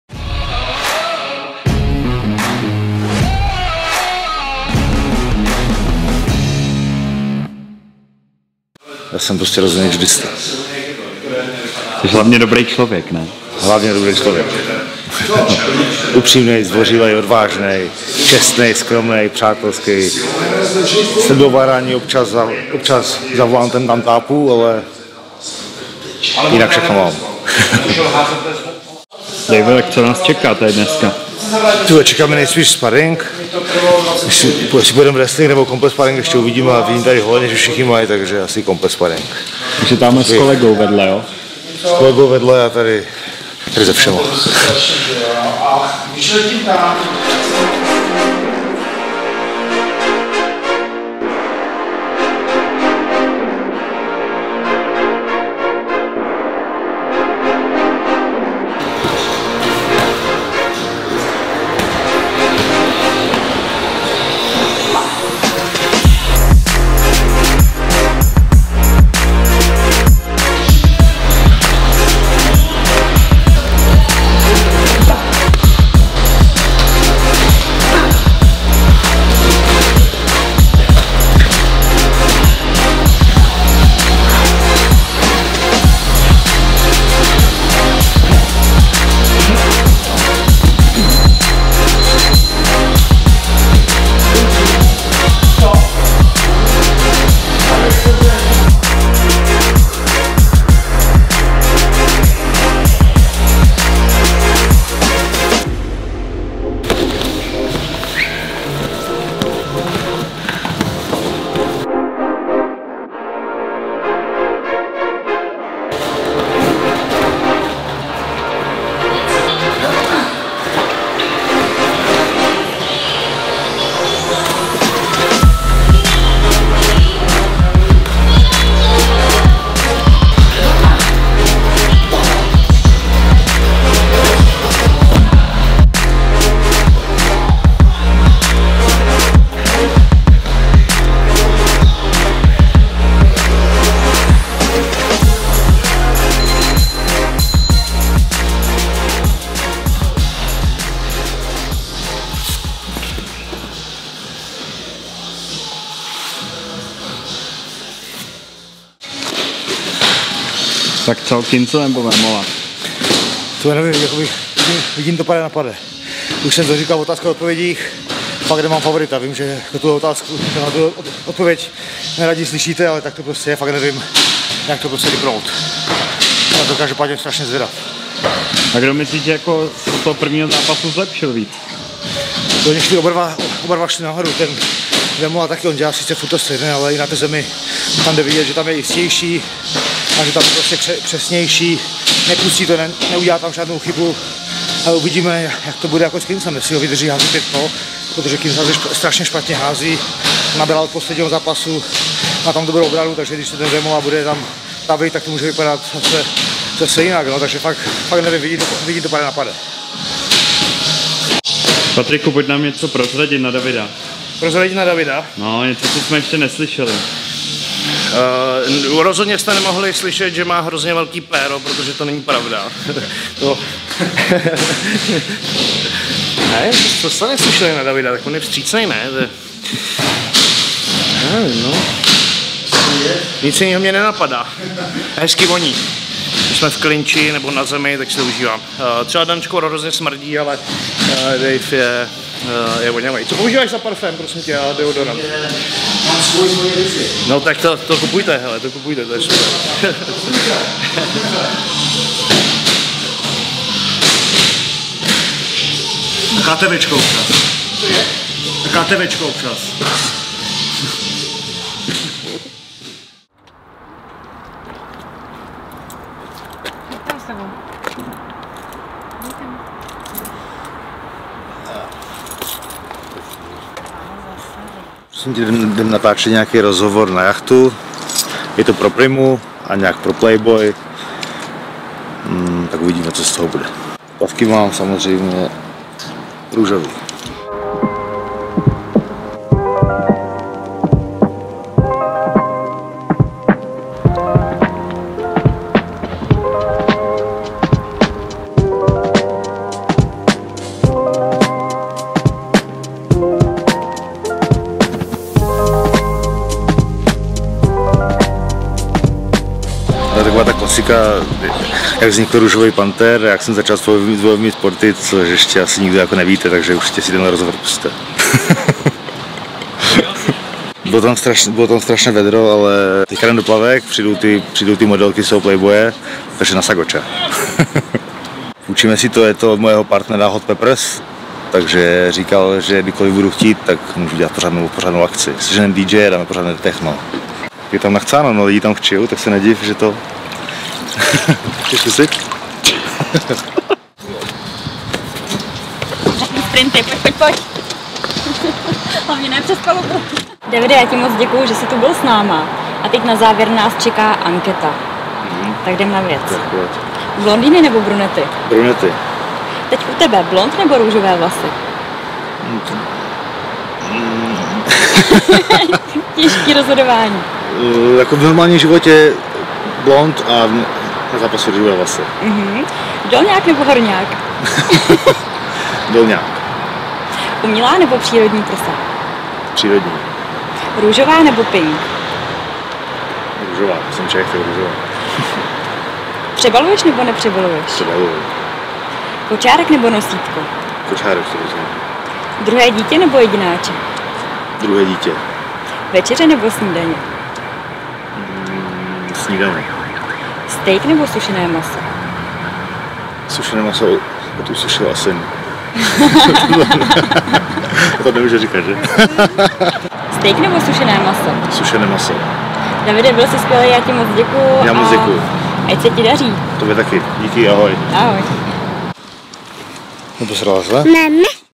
David Kozma | #9 Vlog. Sparingy s Kinclem. Zákulisí natáčení pro PRIMA COOL.
Nezapomeňte sledovat na Instagramu:
@david.pinkpanther.kozma /champ
@dan.wertheim / kamera, střih
@promolabcz / marketing, produkce